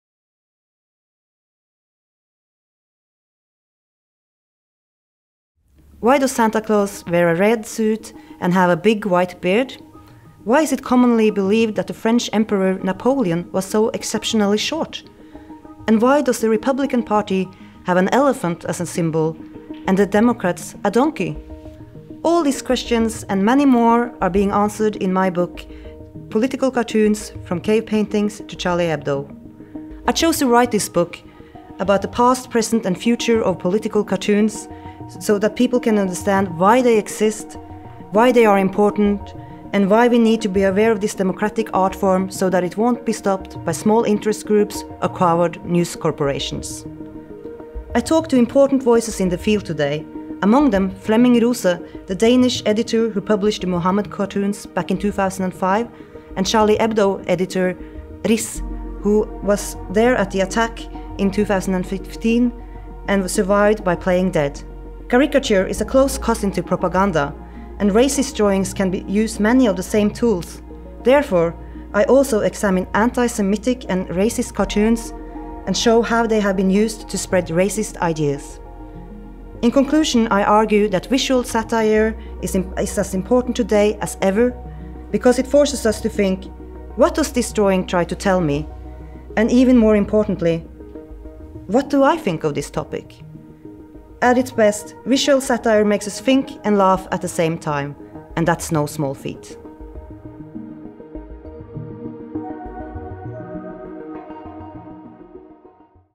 THE HISTORY OF POLITICAL CARTOONS By Erle Marie Sørheim
THE HISTORY OF POLITICAL CARTOONS. FROM CAVE PAINTINGS TO CHARLIE HEBDO By Erle Marie Sørheim
Original title: Karikaturenes historie. Fra hulemalerier til Charlie Hebdo
Genre: Non-fiction
177 pages
Publisher and agent: Humanist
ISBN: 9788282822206
Copyright: Erle Marie Sørheim
Subtitles: Hilde Jørgensen

The presentation of this book is one of eight selected Norwegian books for non-fiction made by NORLA, spring 2022. The films will be presented in webinars for international publishers and translators. The film is also shared with Norwegian embassies all around the world, to reach even further.